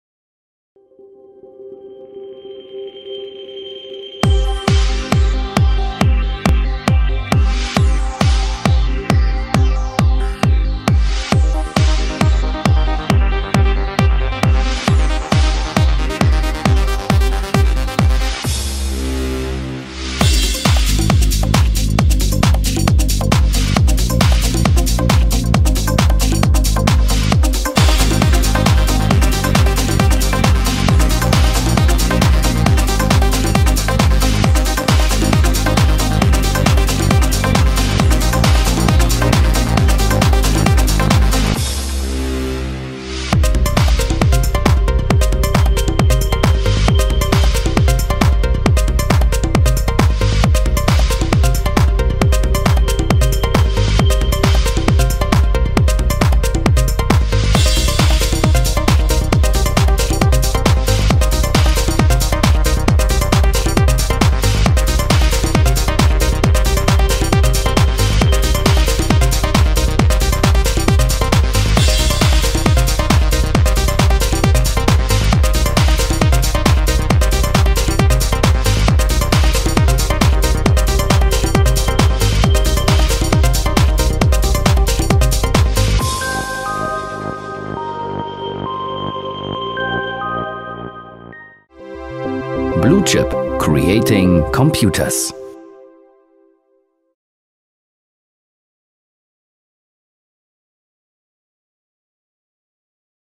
bluechip TRAVELline T10 Tablet
Das neue bluechip 10,1"-Tablet TRAVELline T10 bietet mit seinem hochauflösenden IPS-Panel (1.920 × 1.200 Pixel) und der 10-Finger-Multi-Touch-Unterstützung die besten Voraussetzungen für komfortables Arbeiten unter Windows 8. Ausgestattet mit leistungsfähiger CPU und 4 GB Arbeitsspeicher eignet es sich auch für ressourcenhungrige Applikationen. Besonders interessant für Unternehmen sind der Fingerprint-Sensor sowie der austauschbare Akku, wodurch dem Anwender eine noch längere mobile Nutzung ermöglicht wird. Im Lieferumfang des Tablets befindet sich ein hochpräziser, aktiver Stift, der z.B. in Verbindung mit der implementierten Handschrifterkennung von Windows 8 oder beim Zeichnen sehr gute Dienste leistet. Die besonderen funktionalen Anforderungen des Businessalltags können durch den Einsatz der optional erhältlichen Docking Station mit integrierter Tastatur, zusätzlicher Batterie, je einem Ethernet-, VGA- und USB-Anschluss sowie einem SDHC-Kartenslot noch flexibler erfüllt werden.